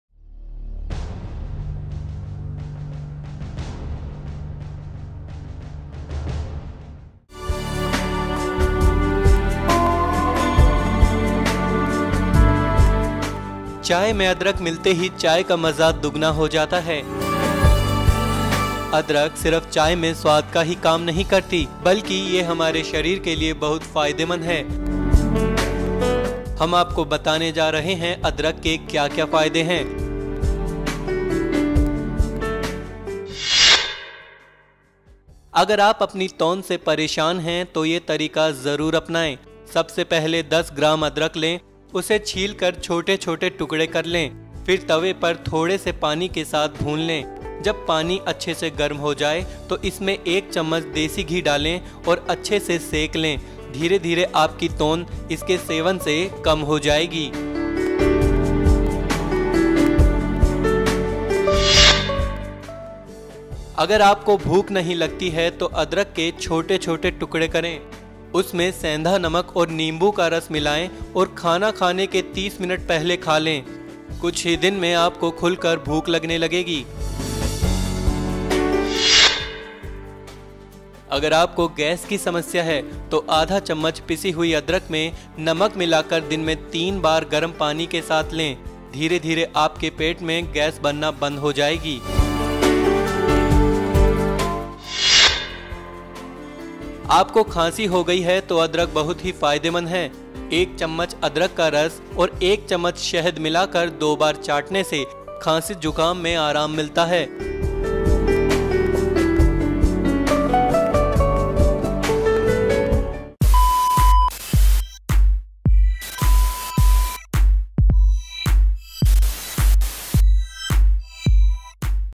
अदरक के रस से दूर होगी तोंद और गैस की Problem, देखें Video
Now a days people are very much concerned about their health and especially their fat. Watch this video to see how you can reduce belly fat!
चाय में अदरक मिलते ही चाय का मजा दुगना हो जाता है। अदरक सिर्फ चाय में स्वाद का ही काम नहीं करती बल्कि ये हमारे शरीर के लिए बहुत फायदेमंद है हम आपको बताने जा रहे हैं अदरक के क्या-क्या फायदे हैं।